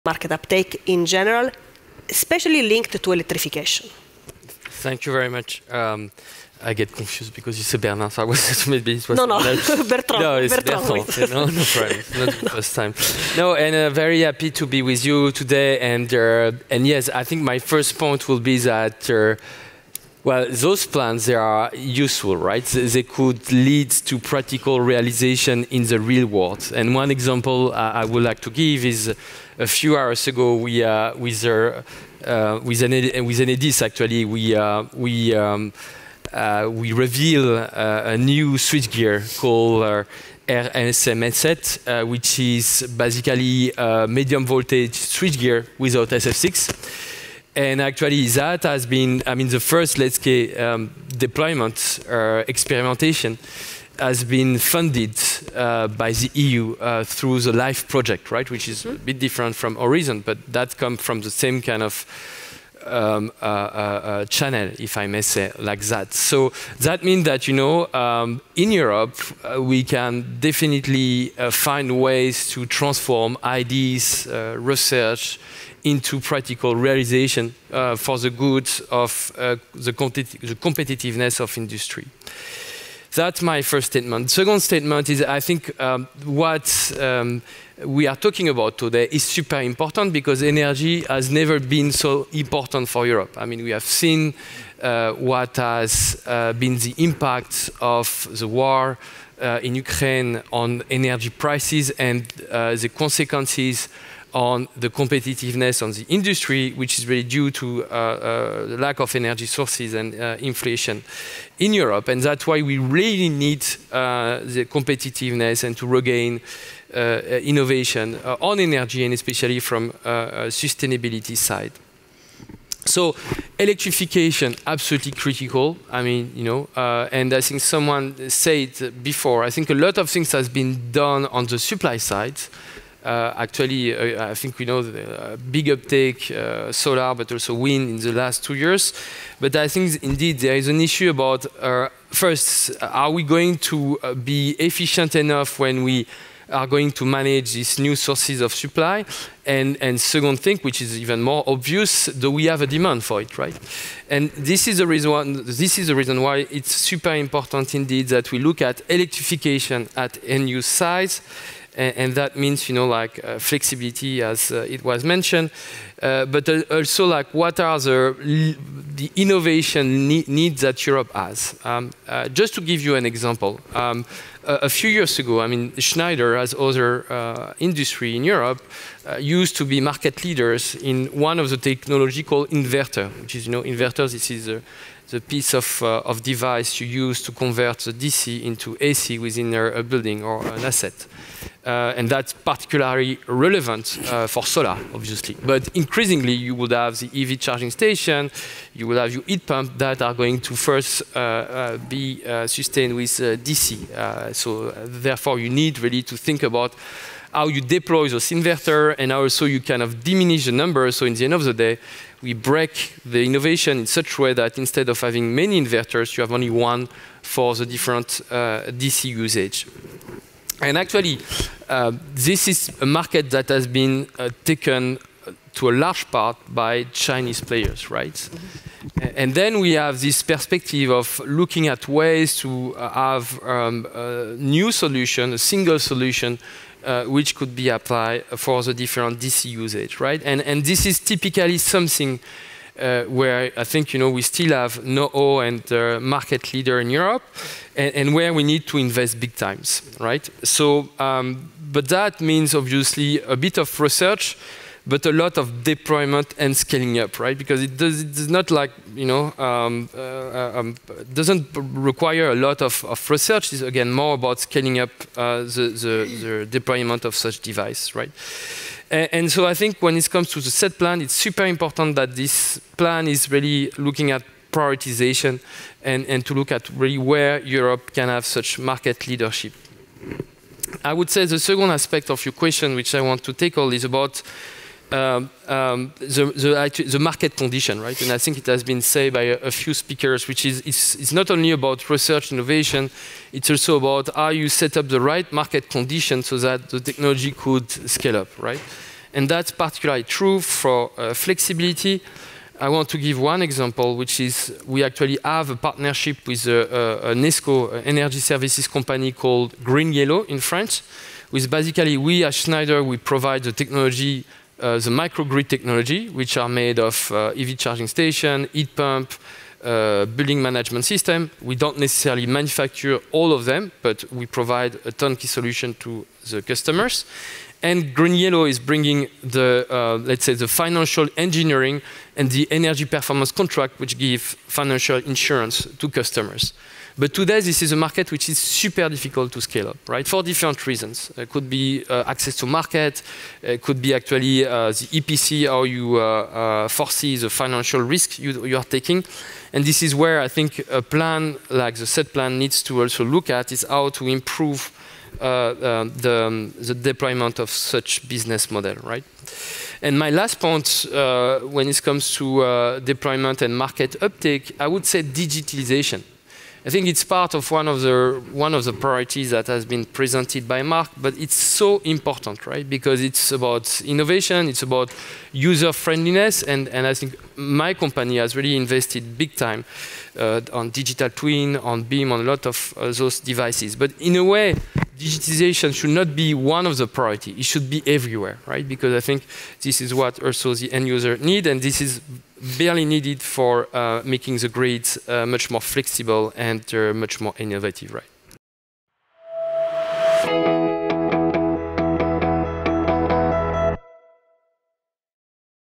Enlit Europe 2023: Bertrand Deprez on grid flexibility | Schneider Electric
Watch the panel discussion featuring Bertrand Deprez, our VP of Europe Government Affairs, where he shares insights on deployment and market uptake related to electrification. He talks about the importance of setting the right market conditions and digitalization to make grids more flexible.

👋 GET TO KNOW US BETTER